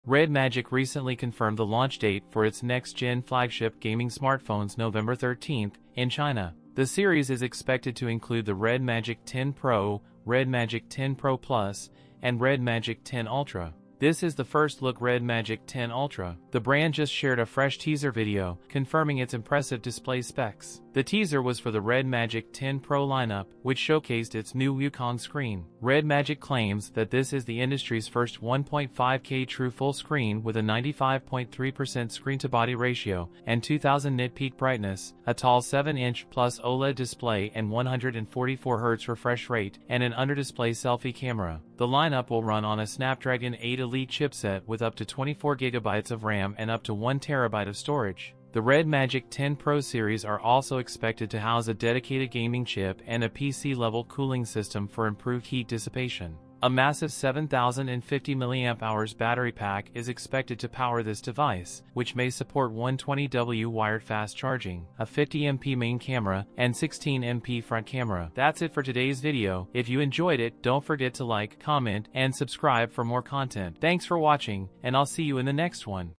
REDMAGIC 10 Pro Unveiled! Official Teaser, Leaks & Price - Game-Changer?
in this video, I like to share with you guys ZTE is preparing a new series of REDMAGIC 10 smartphones, Red Magic recently confirmed the launch date for its next gen flagship gaming smartphones November 13, in China. The series is expected to include the Red Magic 10 Pro, Red Magic 10 Pro+, and Red Magic 10 Ultra,
The lineup will run on a Snapdragon 8 Elite chipset with up to 24GB of RAM and up to 1TB of storage, a 50MP main camera, and 16MP front camera. I hope you guys like and enjoy it.